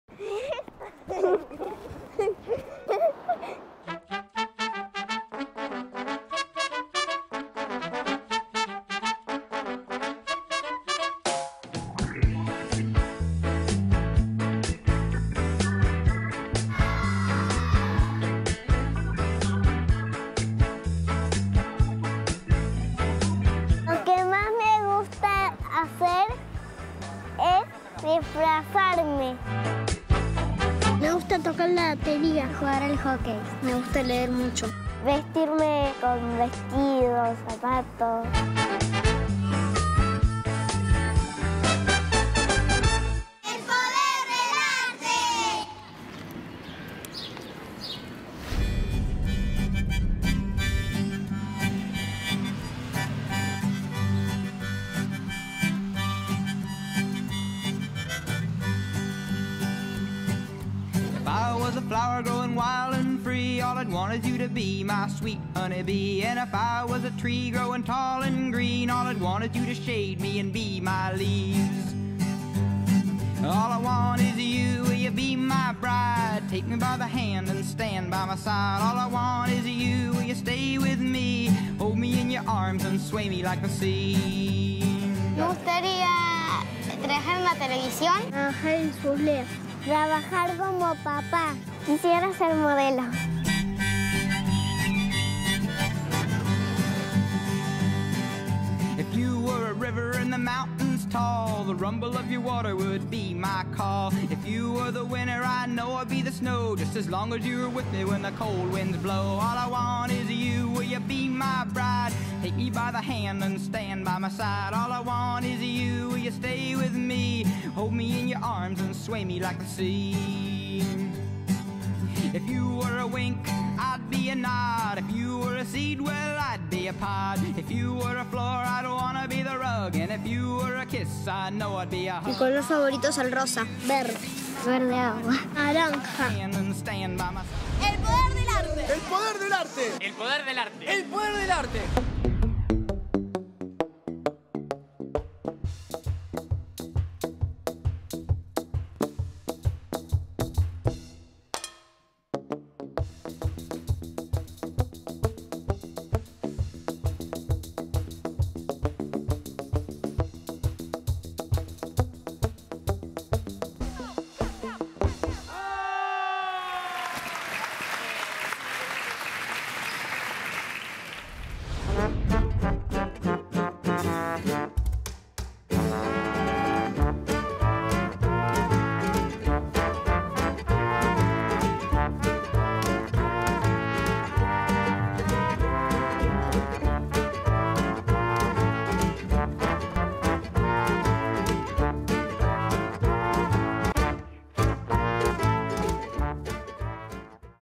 Sullair Argentina l Mejor encontrar(nos) 2012 l El Poder del Arte.
Festejamos el día de la familia junto a todos los hijos de nuestros empleados. Disfrutamos de un lindo momento acercándole a los niños distintas ramas del arte para que puedan participar y experimentar propuestas divertidas!